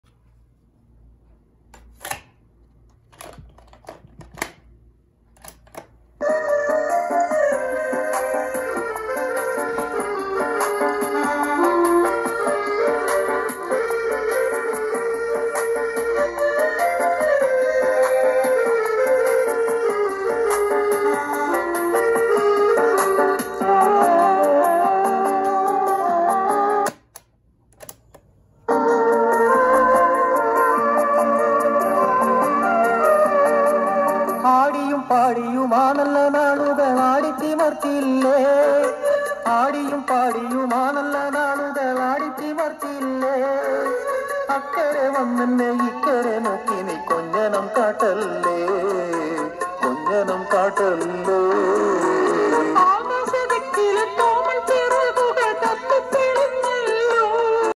Tape Testing | Old is Gold Mappila Songs Collections
ഇവിടെ കറണ്ട് പോയതാണ് , ഉണ്ടായിരുന്ന ബാറ്റെറിയും തീർന്നു പോയി.. ക്ഷമിക്കുമല്ലോ
Old is Gold Mappila Songs Collections